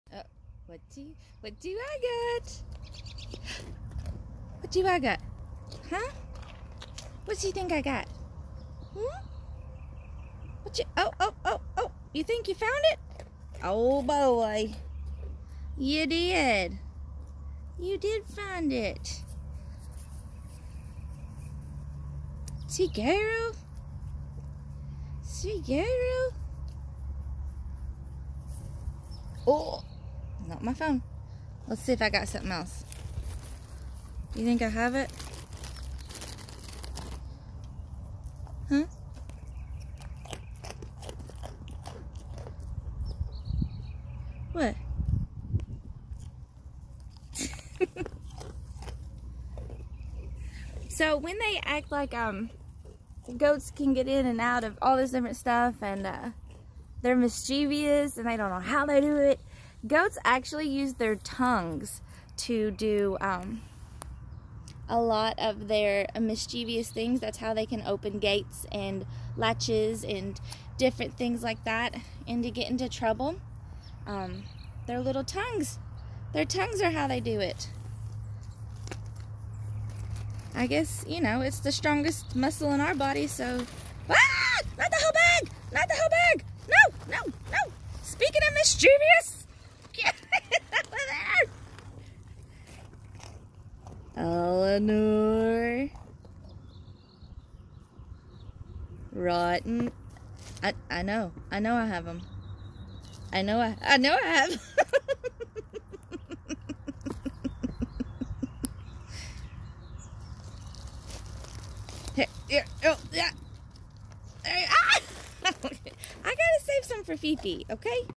Me and Eleanor like to play a game...
I like to pretend like she doesn't know exactly where I have the animal crackers. Spoiler alert: she ALWAYS knows!